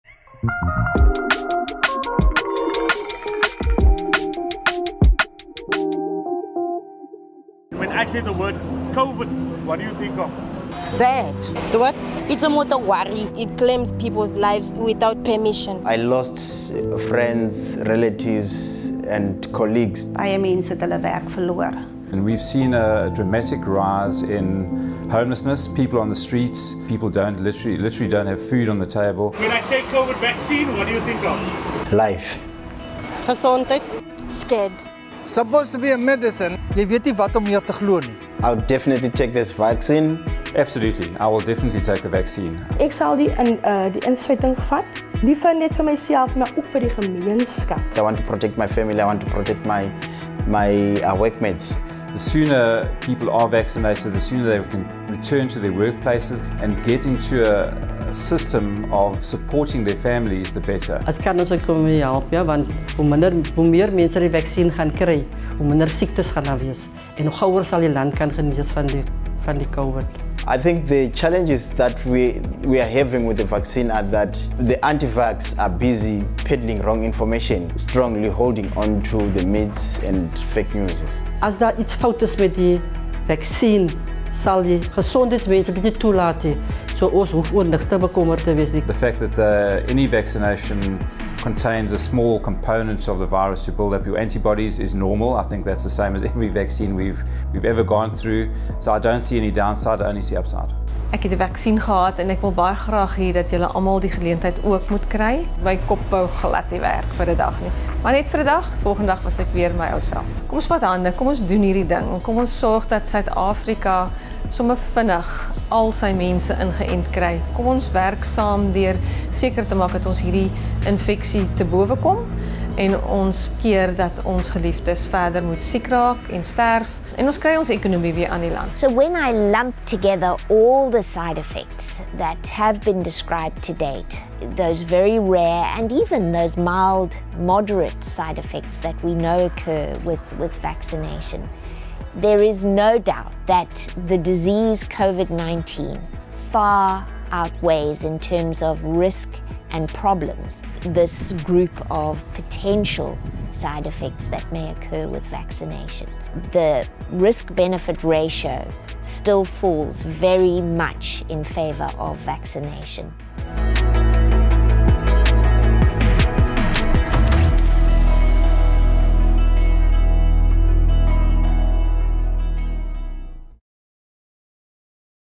Benefits vs Side effects: Bellville
Do the benefits of the vaccine outweigh the side effects? Take a look at what community leaders, scientists, and health workers in Bellville have to say. Get informed with accurate information to make the right choice for you! to register for the vaccine online.